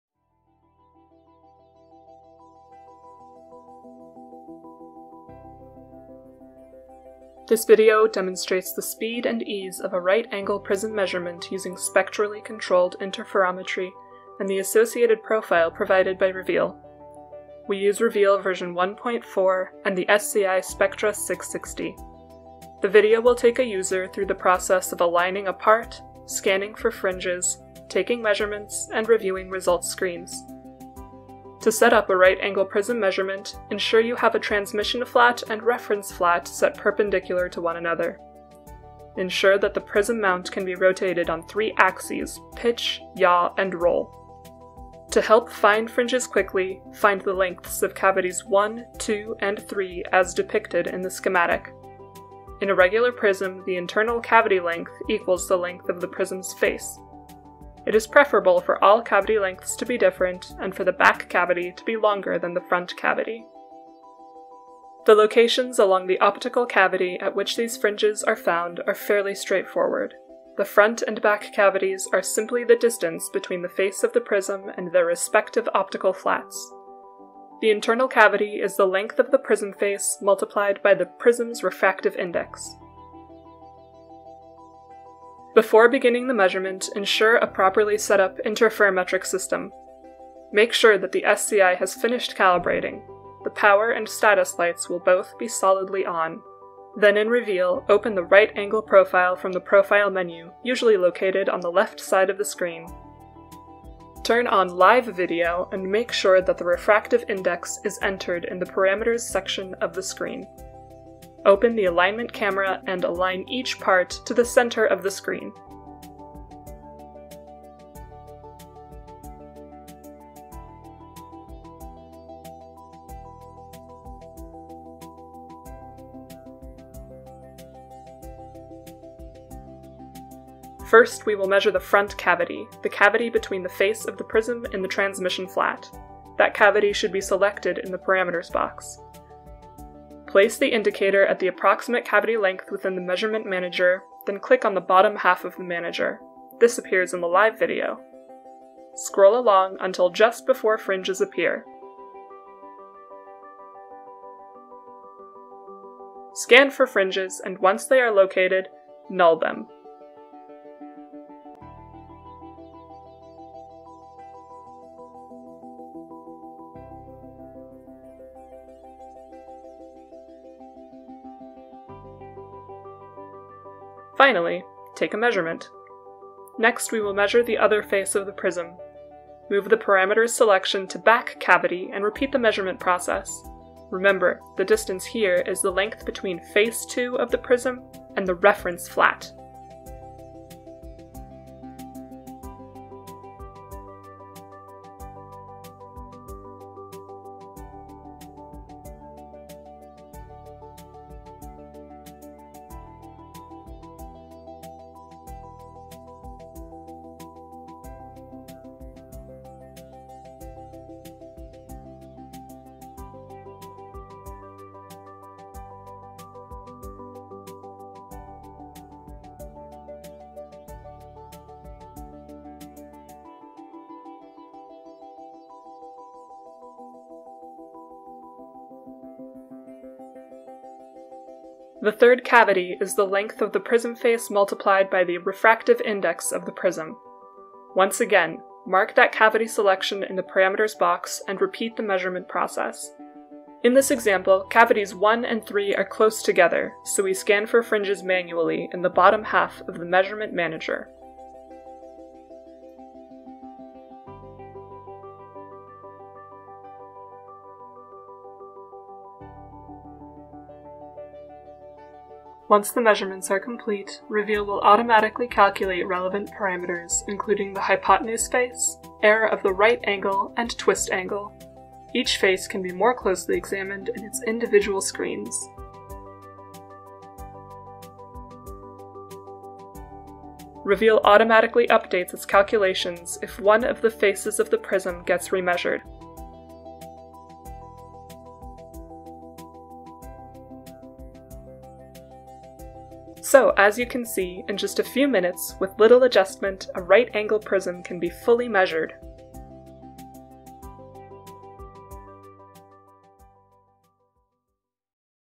Fast & Accurate Spectrally Controlled Interferometry (SCI) Prism Measurement
Watch the fast measurement of a right-angle prism using APRE’s Spectrally Controlled Interferometry (SCI). The best part is that there is no preparation required.
Rapidly measure and report on:
• Front Face
• Back Face
• Hypothenuse
• Transmitted Wavefront